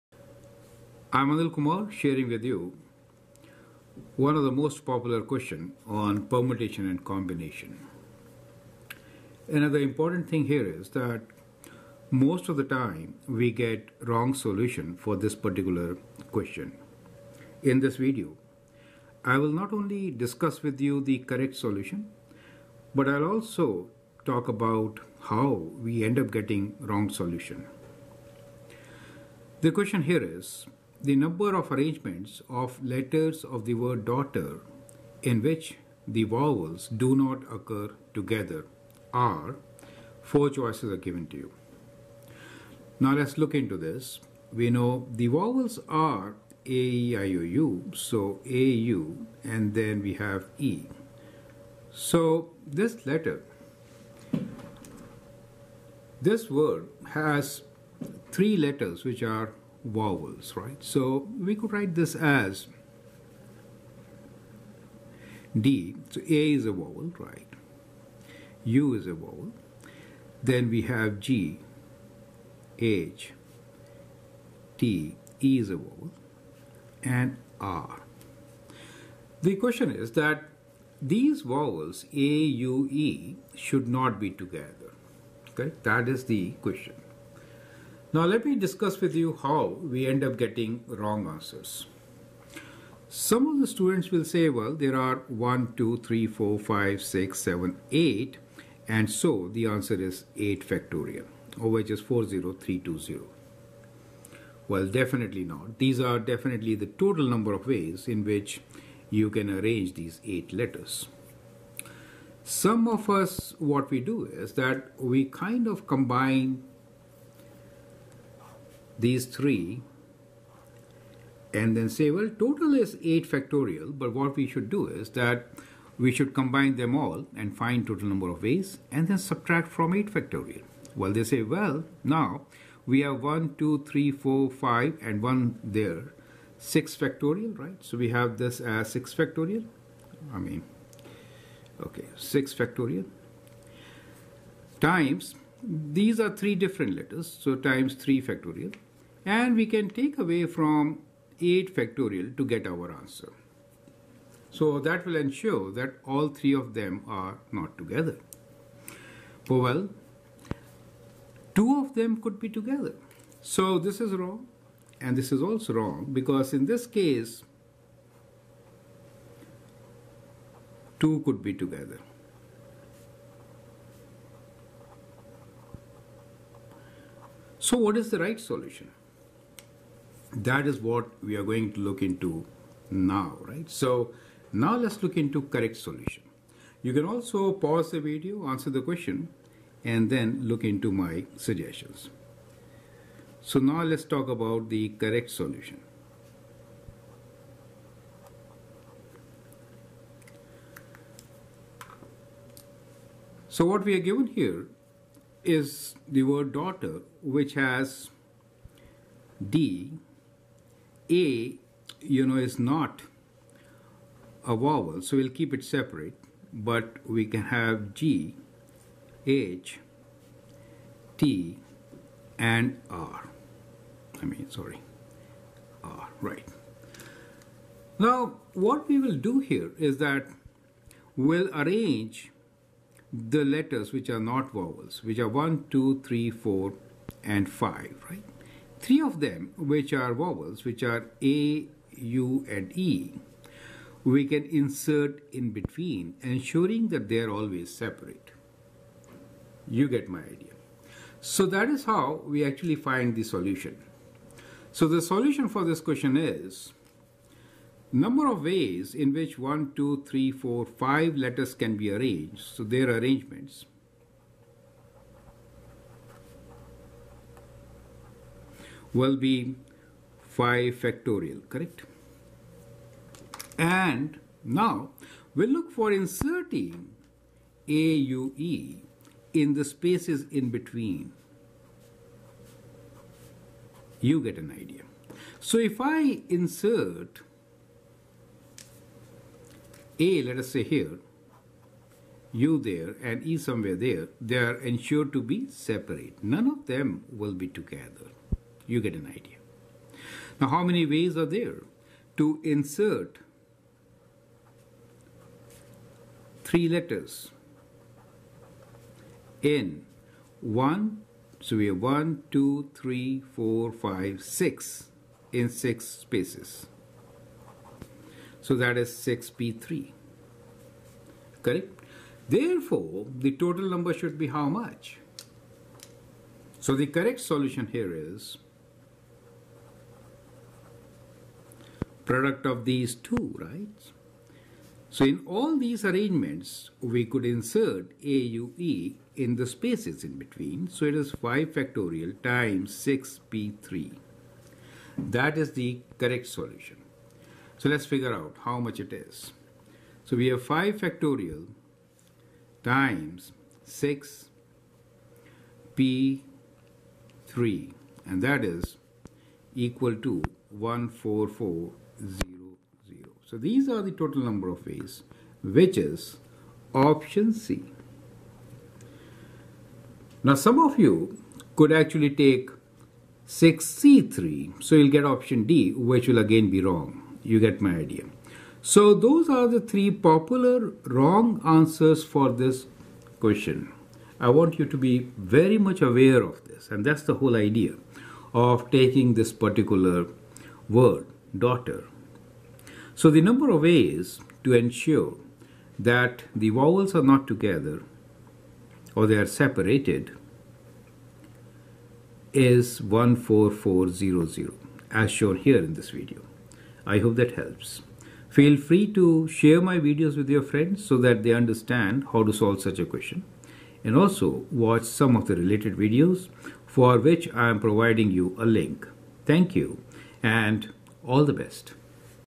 Number of Word where vowels are not together in the word DAUGHTER only Correct Solution
The number of arrangements of letters of the word DAUGHTER in which the vowels do not occur together are:
A 40320
B 36000
C 14400
D 2400
Combinations with Some Identical Items is an important concept which has been discussed with easy to understand approach. The number of combinations with p similar items is p + 1. The multiplicative rule is applied to derive a general solution.
Organized Counting
Fundamentals of Counting Principles
Permutations and Factorials